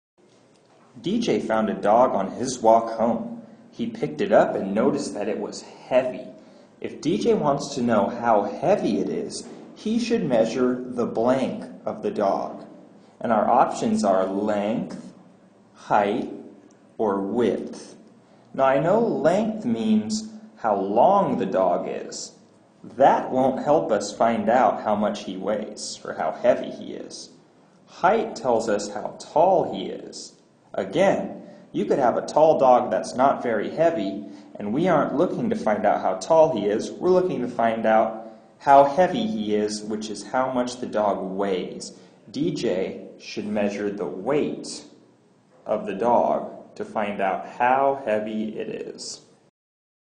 [K.MD.1-1.2] Concept of Measurement - Common Core Standard
Understand that objects can be measured in different ways, for example they can be weighed, measured for length, measured for size, etc.